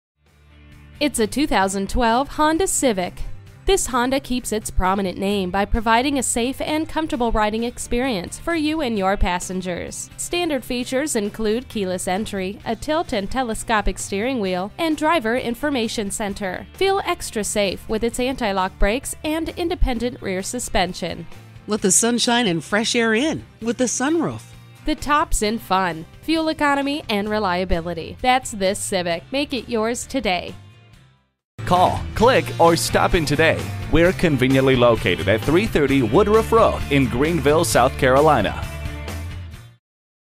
2012 Honda Civic Greenville SC Easley, SC 150363A - SOLD
Year: 2012
Make: Honda
Model: Civic
Trim: EX
Engine: 4 Cylinder
Transmission: 5 Speed Automatic
Color: Alabaster Silver Metallic
Interior: Gray
Mileage: 41335
Stock #: 150363A
VIN: 2HGFB2F84CH603628
HONDA CERTIFIED***7 YEAR/100K WARRANTY***BUY WITH CONFIDENCE!!, CLEAN CARFAX! ONE OWNER!, And SUNROOF/MOONROOF!. Air Conditioning, Alloy wheels, Dual front side impact airbags, Reclining Front Bucket Seats, Remote keyless entry, and Speed control. Creampuff! This handsome 2012 Honda Civic is not going to disappoint. There you have it, short and sweet! Honda Certified Pre-Owned means you not only get the reassurance of a 12mo/12,000 mile limited warranty, but also up to a 7yr/100k mile powertrain warranty, a 182-point inspection/reconditioning. Honda has established itself as a name associated with quality. This Honda Civic will get you where you need to go for many years to come. The 2012 Civic received the highest possible safety rating from the Insurance Institute for Highway Safety.

Address:
330 Woodruff Rd